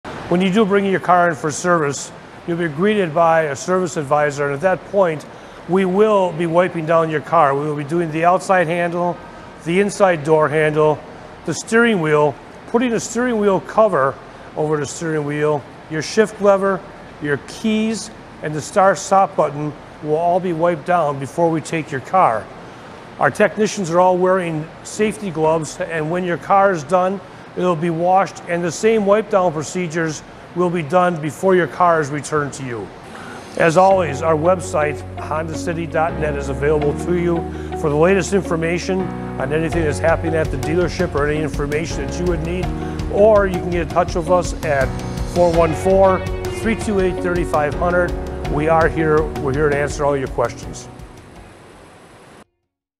Auto Servicing | Cleaning Tips: Wipe Down & Disinfecting Your Car | COVID19 Precautions | Honda City
Cleaning Tips: Wipe Down and Disinfecting Your Car | COVID19 Precautions

We Want To Keep You Safe and Comfortable When Servicing Your Vehicle
If you are uncomfortable entering the dealership you may use our night drop box at any time, no appointment necessary. Just fill out an envelope and drop it through the mail slot located on door #1 (We will continue to disinfect the mail drop box lid, please bring your own pen).

We wipe down and disinfect the following items Before AND After Servicing:
 * Outside Door Handle
 * Key or Start/Stop Button
 * Key Fob
 * Inside Door Handle
 * Steering Wheel
 * Shift Lever/Push Button Transmission

When Service is complete just let us know when you would like to pick-up your vehicle and we can have it parked outside. A link can be sent to your email/phone for secure online payment.